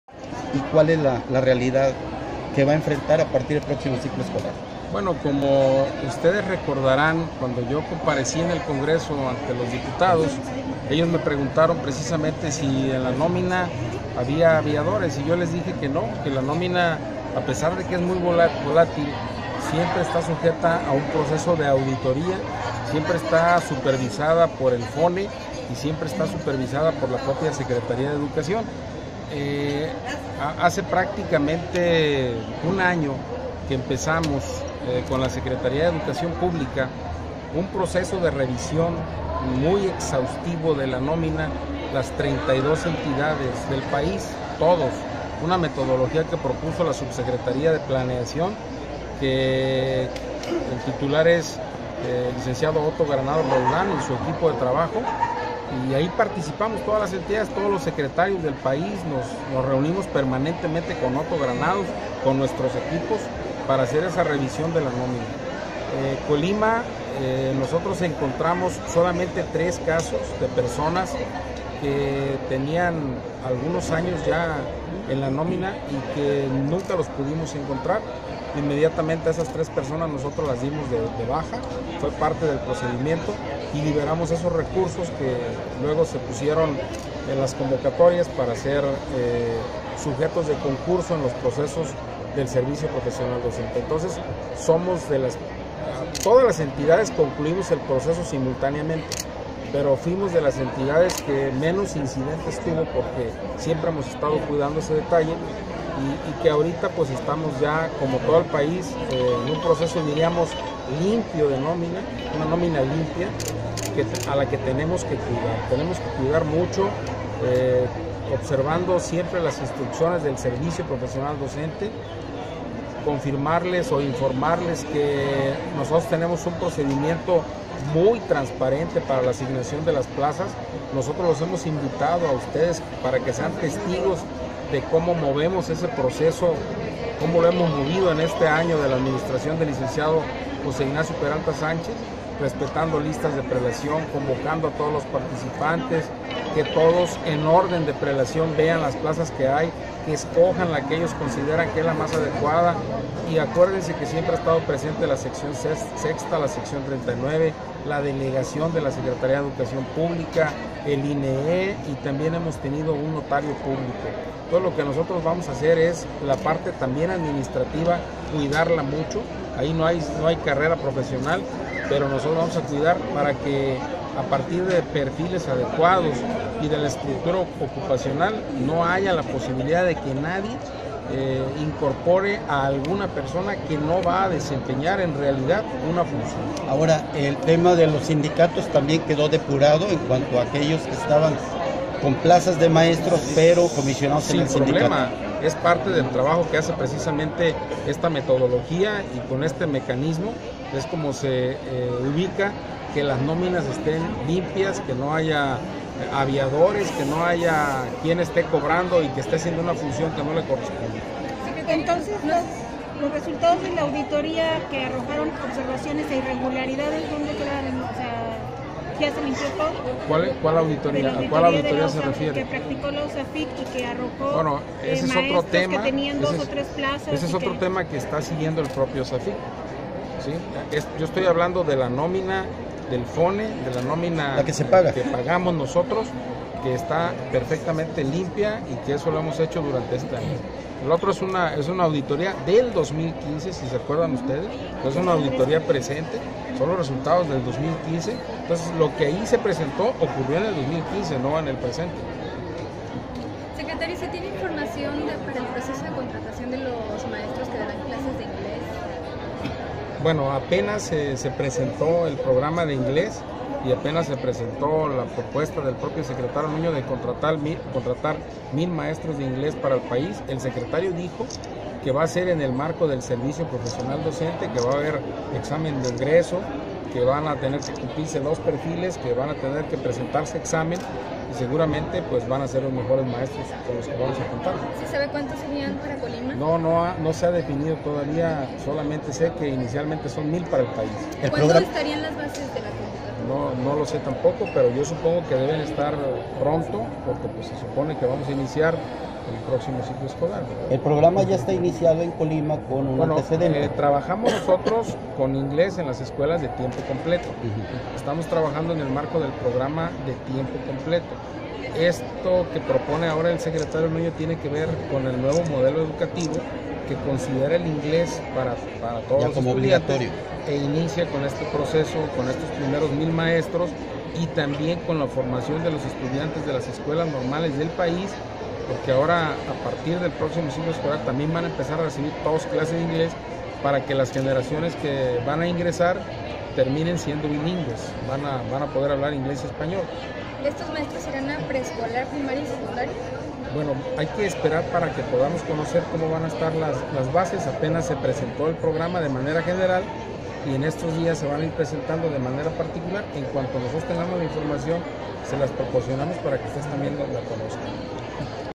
OSCAR JAVIER HERNÁNDEZ ROSAS, SECRETARIO DE EDUCACIÓN DEL GOBIERNO DEL ESTADO DE COLIMA
No hay 'aviadores' en Secretaría de Educación, tenemos una nómina limpia: Hernández Rosas